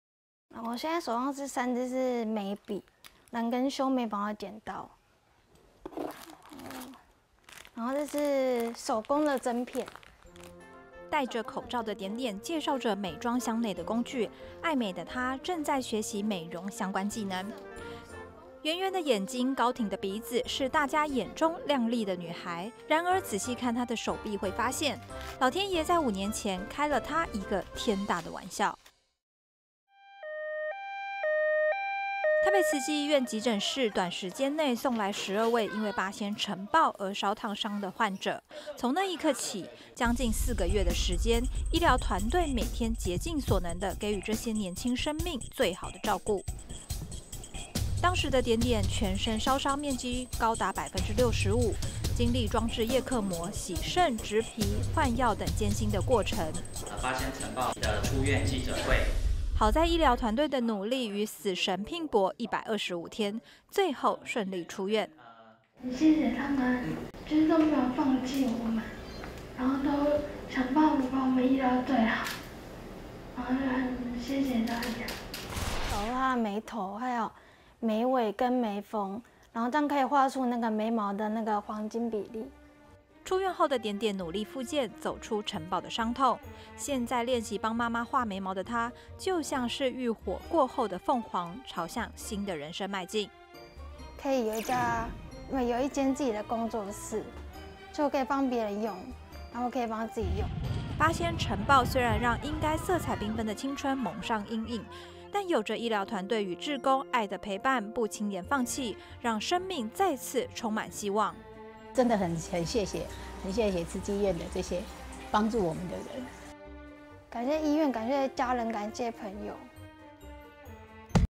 從火燄裡歸來 點點的彩妝人生
北慈15陪伴，這是走出八仙塵爆重生的故事。

八仙塵爆傷患 點點：「我現在手上這三隻是，眉筆跟修眉毛的剪刀，然後這是手工的針片。」

帶著口罩的點點，介紹著美妝箱內的工具，愛美的她正在學習美容相關技能。

圓圓的眼睛高挺的鼻子，是大家眼中亮麗的女孩。然而，仔細看她的手臂會發現，老天爺在五年前她開了一個天大的玩笑。

台北慈濟醫院急診室短時間內，送來12位因為八仙塵暴而燒燙傷的病患。從那一刻起將近四個月的時間，醫療團隊每天竭盡所能地給予這些年輕生命最好的照顧。

當時的點點全身燒傷面積達65%，經歷裝置葉克膜、洗腎、植皮、換藥等艱辛的過程。好在醫療團隊的努力與死神拚搏125天，最後順利出院。

八仙塵爆傷患 點點：「謝謝他們都沒有放棄我們，都想辦法幫我醫療到最好 ，很謝謝大家 。」

八仙塵爆傷患 點點：「找出她的眉尾跟眉鋒，這樣可以畫出眉毛的黃金比例。」

出院後的點點努力復健，走出塵暴的傷痛。現在練習幫媽媽畫眉毛的她就像是浴火過後的鳳凰，朝新的人生邁進。

八仙塵暴傷患 點點：「可以有一間自己的工作室，可以幫別人用也可以幫自己用。」

八仙塵暴雖然讓應該色彩繽紛的青春蒙上陰影，但有著醫療團隊與志工，愛的陪伴、不輕言放棄，讓生命再次充滿希望。

八仙塵暴傷患家屬 點點媽：「真的很謝謝慈濟醫院這些幫助我們的人。」

八仙塵爆傷患 點點：「感謝醫院、感謝家人，感謝朋友的陪伴。」

企畫採訪 黃欣妍
攝影剪輯 郭家旭